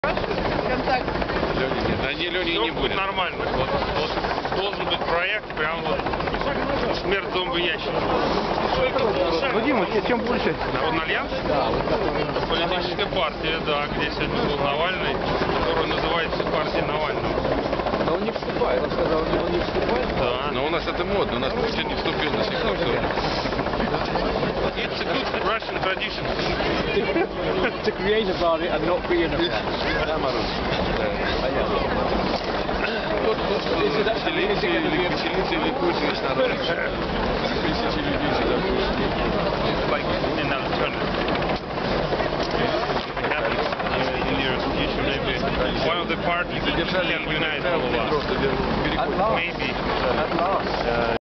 Гудковы и Герой России.Москва,Лубянка 15.дек.2012
Дима Гудков и Герой России Сергей Нефедов.Москва,Лубянка,,Соловецкий Камень 15-е.декабря.2012